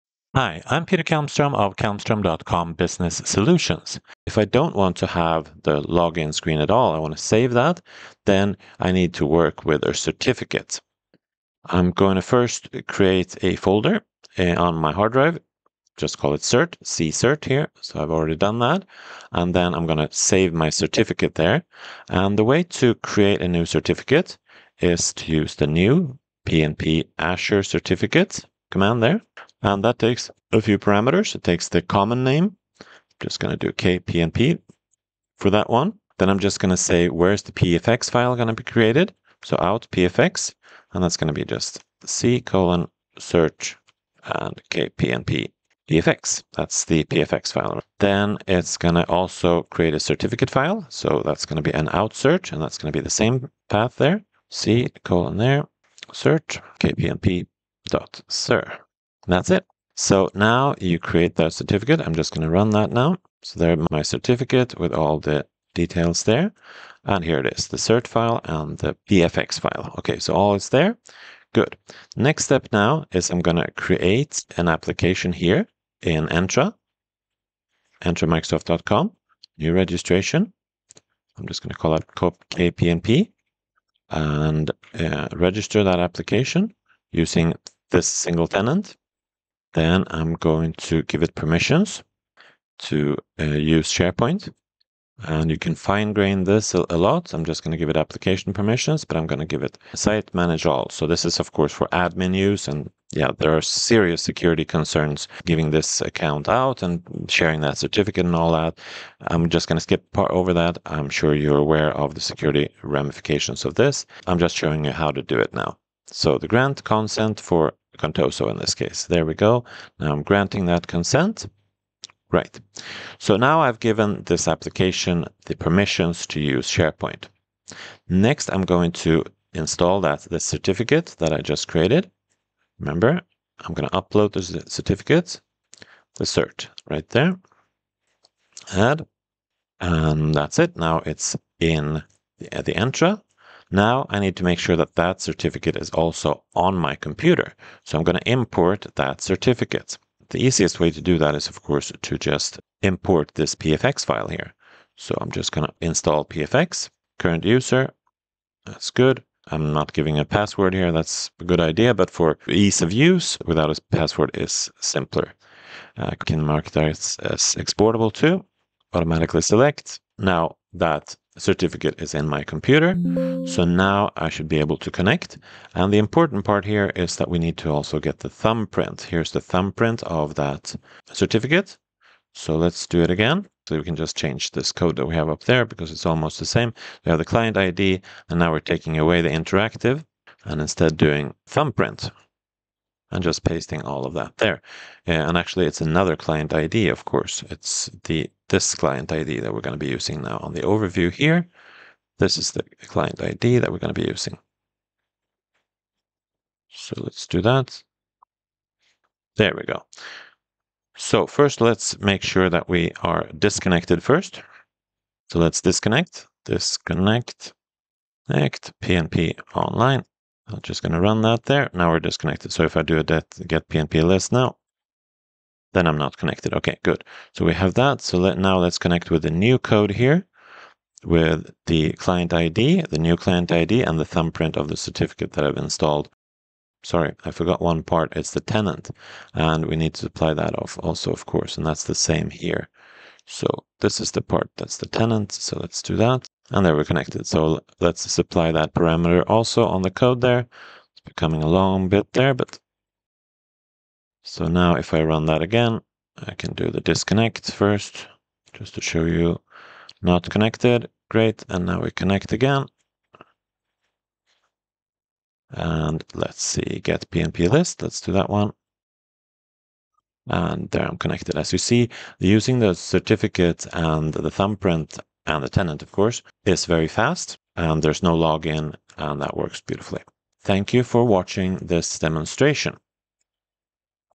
Connect PowerShell to SharePoint with a Certificate
Use an MS Entra registered certificate that allows SharePoint modifications without authentication